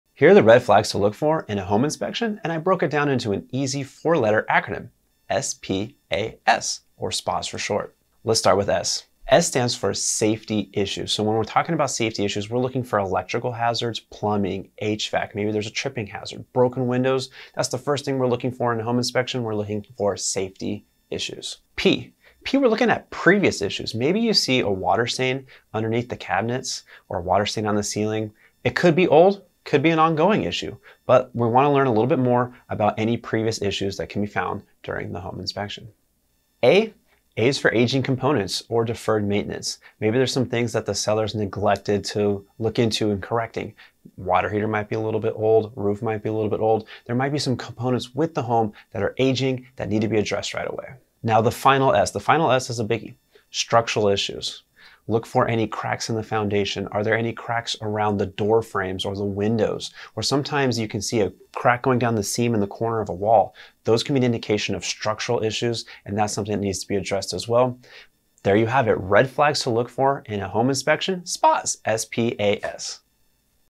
Red Flags to Look for in a Home Inspection - SPAS
Red flags 🚩 to look for in a home inspection. An easy acronym to help you remember: SPAS ⤵️

S - Safety Issues: Issues such as electrical hazards, plumbing, HVAC, tripping hazards, broken windows, etc

P - Previous Issues: Look for potential previous issues such as stains on the ceiling, walls, under the cabinet, any patching…

A - Aging Components: These are your potential deferred maintenance issues. Is the roof old, is the water heater on its last days, or maybe the home you’re buying has the first-ever air conditioning unit! Whatever it is, you want to understand what potential issues you may face immediately due to age.

S - Structural Issues: Last but certainly not least. Look for cracks in the foundation, sloping, cracks along the window seals, door jams, or in the corners of the walls. If the home is built on a raised foundation, make sure your home inspector is checking everything out underneath.

I hope this little acronym of SPAS helps you remember the red flags to look for in a home inspection.

Lane Stone | Sackin-Stone Team | Seven Gables Real Estate DRE 01912264